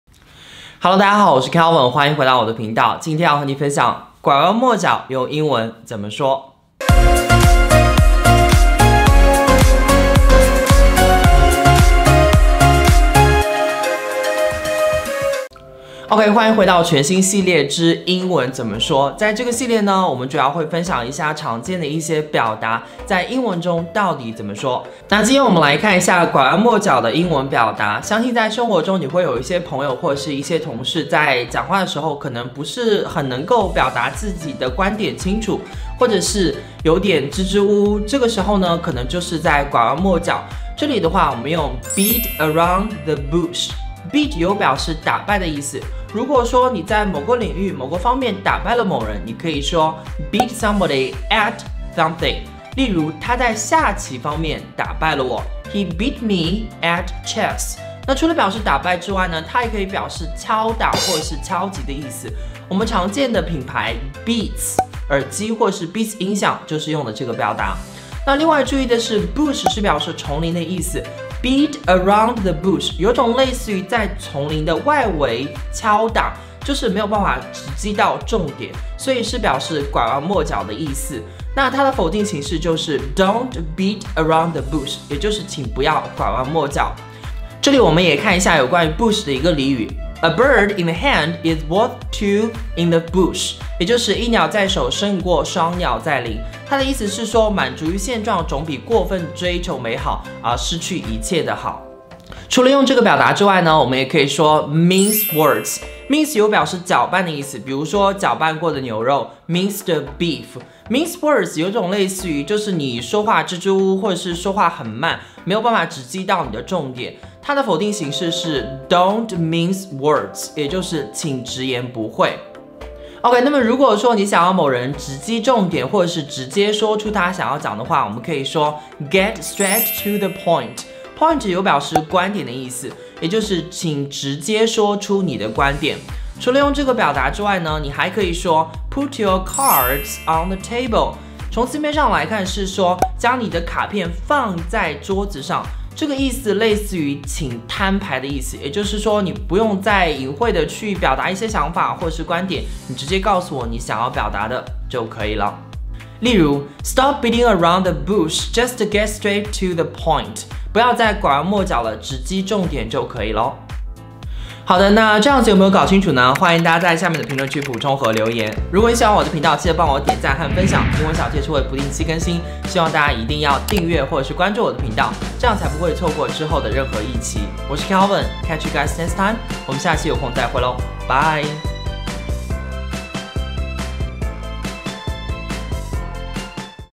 “拐弯抹角”用英文怎么说？
生活中很多时候我们会拐弯抹角的去表达我们的观点，本期和你分享如何用英文说"拐弯抹角”？❤️